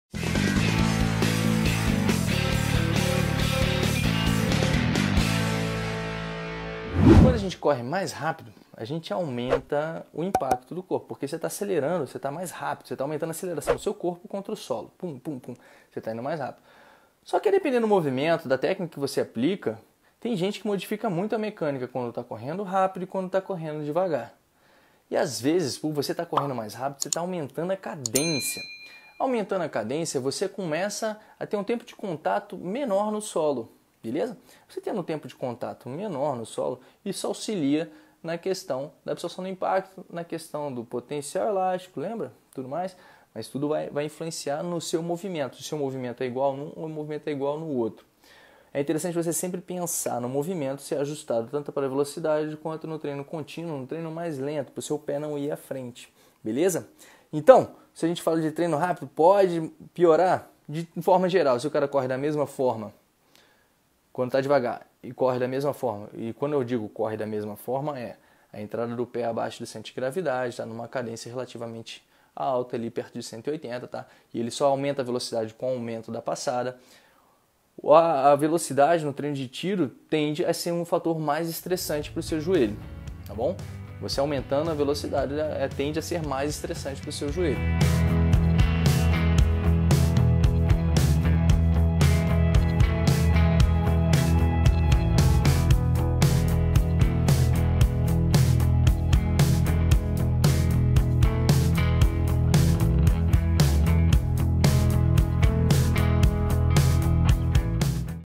Correr mais rápido prejudica o joelho?
Correr mais rápido causa mais impacto nos joelhos? Qual a relação da velocidade com a mecânica da corrida? Você vai encontrar dicas e respostas nesse vídeo rápido. Assista agora!

Acesse a versão gratuita do Corrida Perfeita e teste hoje mesmo uma maneira eficiente de correr livre de dores e lesões